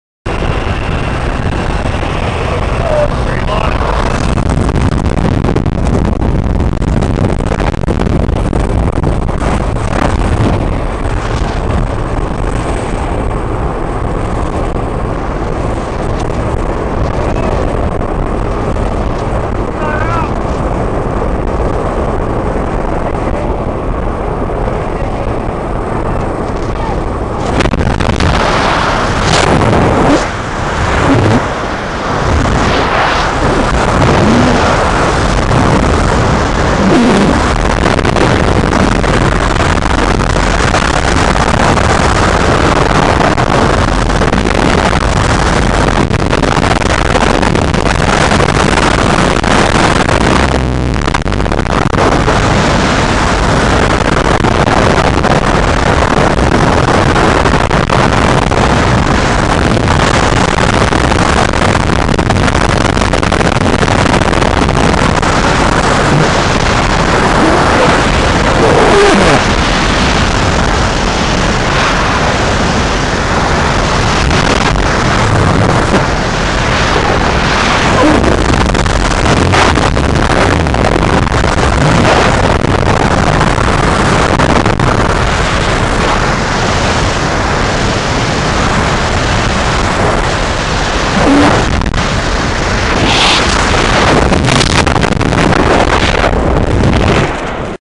Taft...05-23-2009...Jim Wilkins...Celebration
This 18 Way dive was in celebration of Jim's 45 years of Skydiving, 40 years of flying jumpers,and 44 years of flying airplanes.Fun was had by all . not everyone got in but it gives us something to work on.It was a privilege to shoot the video and I'm sure glad the camera worked.